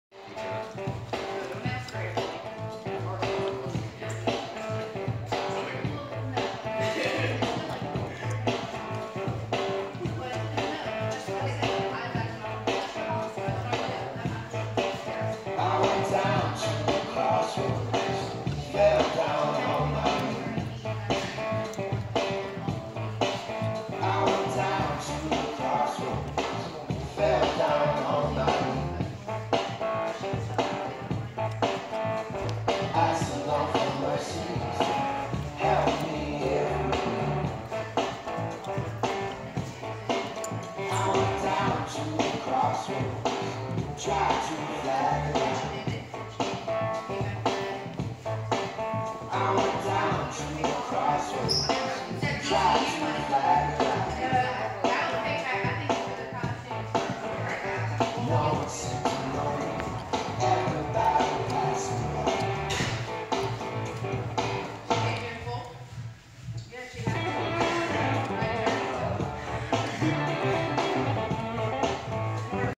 West Coast Swing Kids 2018
Anna dancing wcs with Coach Byron Calix